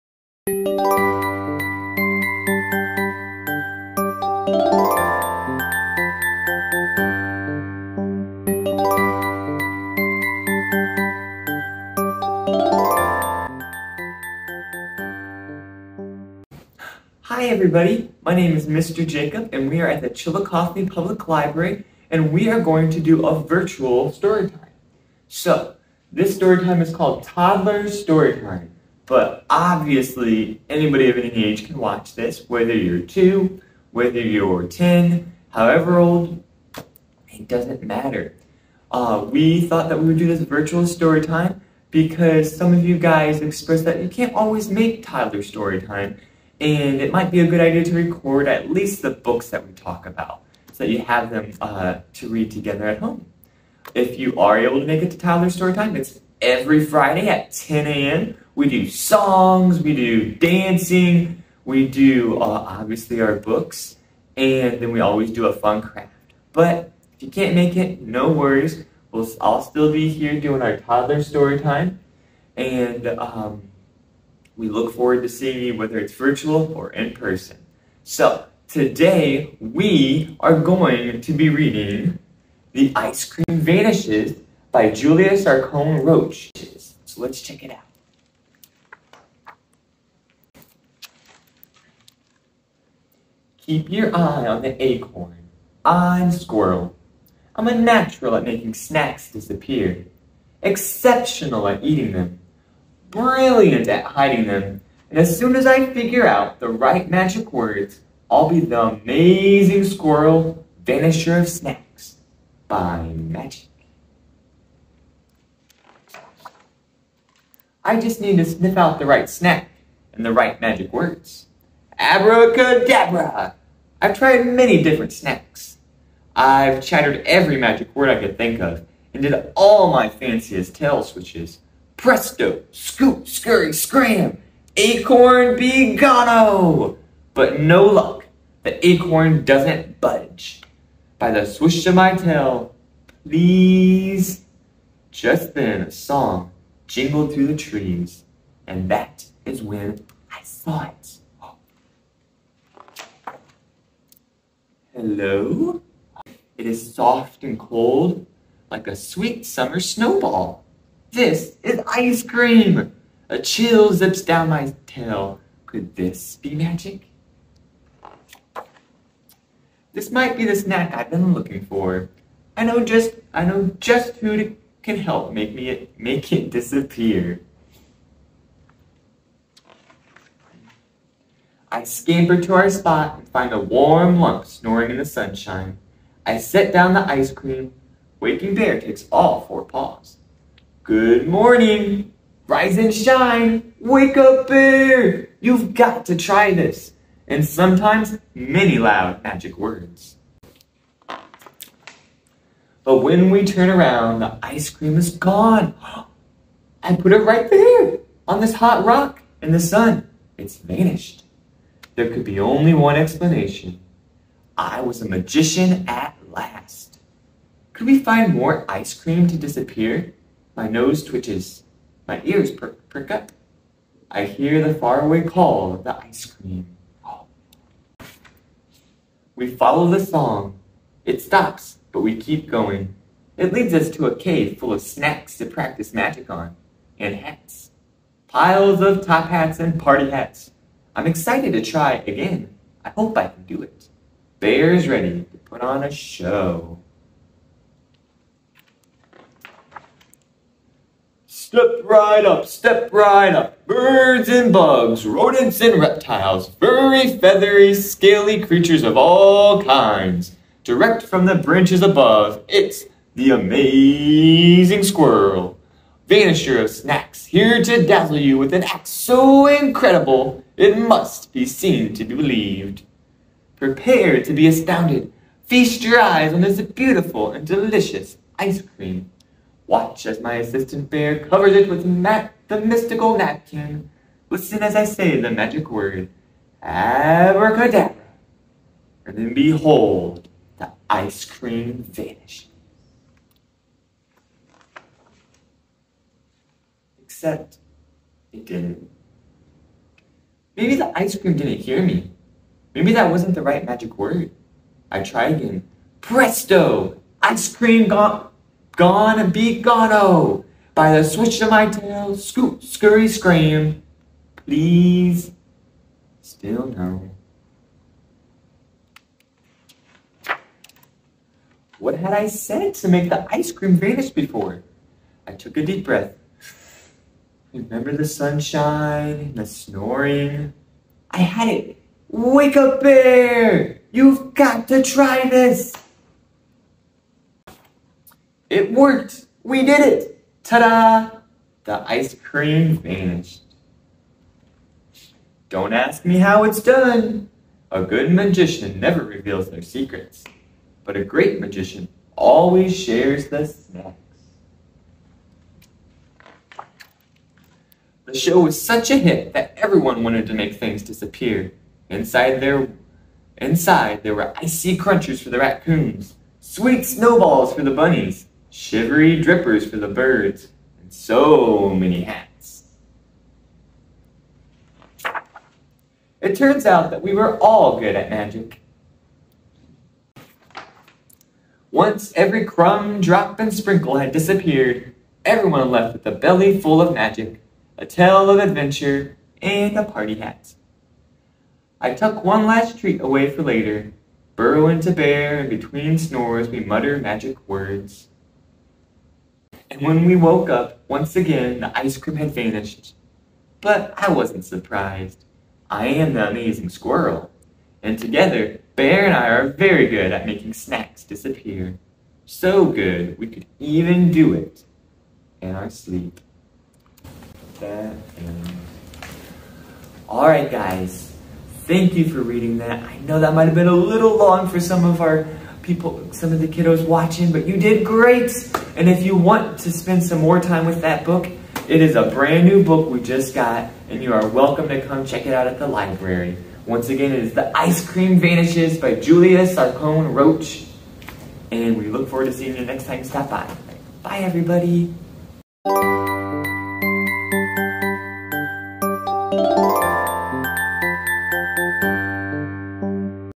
The Ice Cream Vanishes - Chillicothe Public Library Storytime
Join Mr. Jacob for Toddler Storytime! He's reading The Ice Cream Vanishes by Julia Sarcone-Roach.